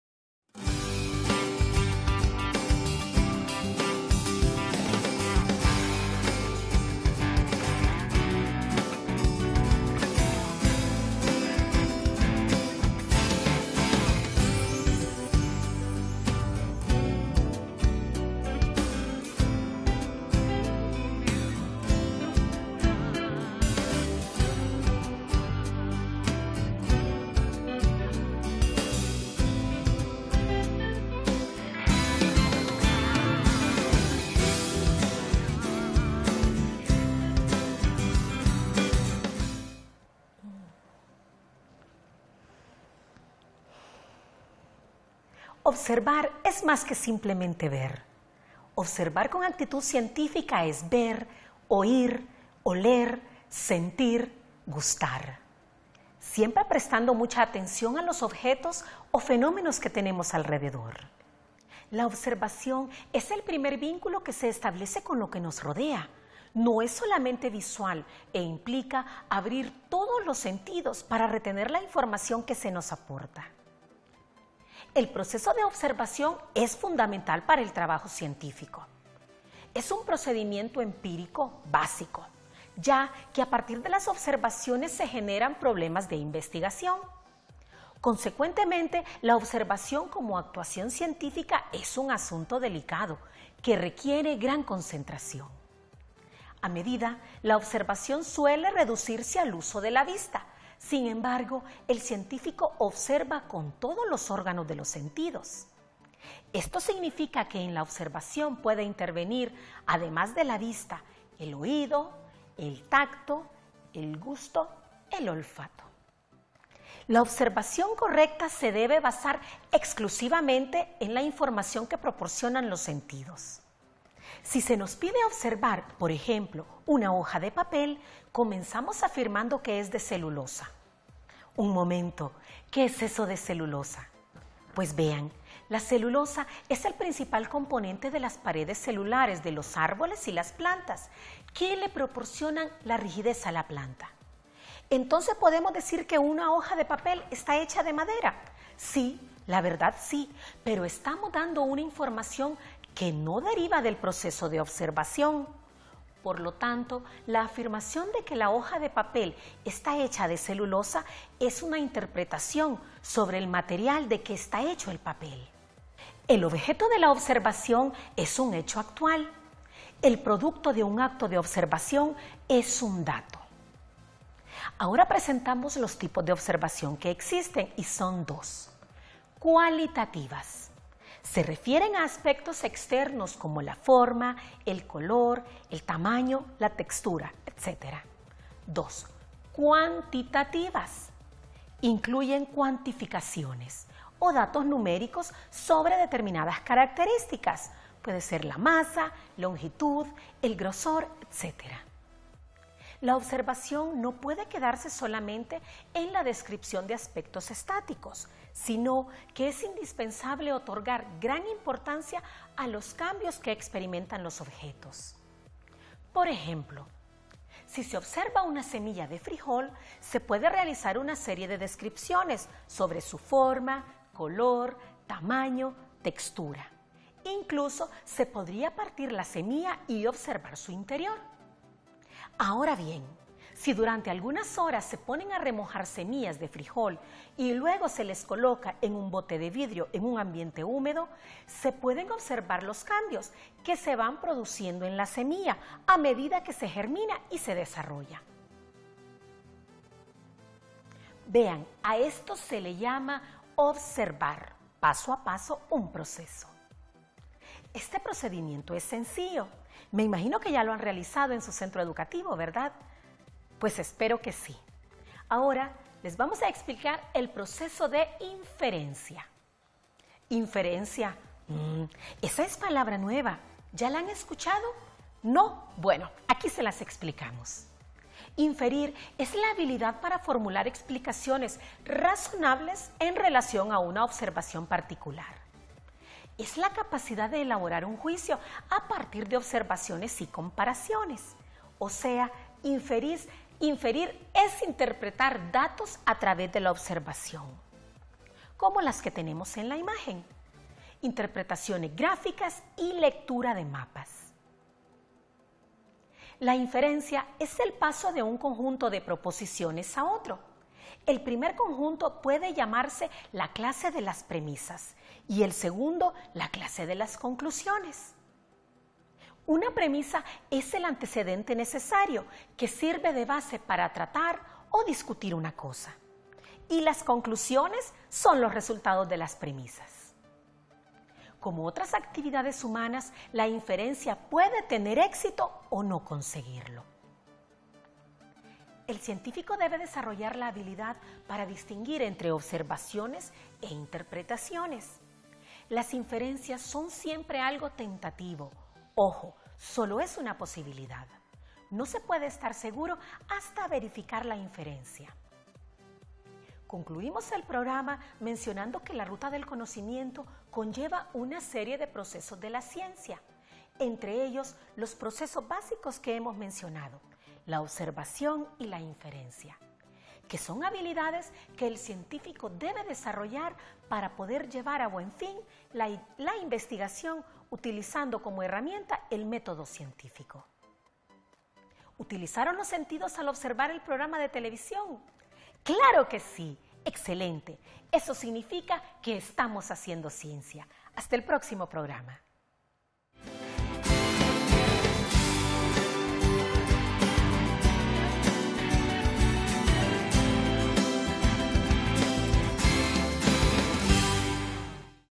#03 Ejercitando los sentidos, 7.º Ciencias Naturales Lección Educativa
Campo del conocimiento: 7.º Grado Ciencias Naturales
Bloque: IV. Materia y energía
Número de Secuencia: 2
Nombre de la Secuencia: ¿Qué hacen los científicos?
Número de programa: 3
Nombre del programa: Ejercitando los sentidos

Recuerda ¡La educación cambia una nación! STVE Telebásica "Transformando la nación con mejor educación" Secretaría de Educación Honduras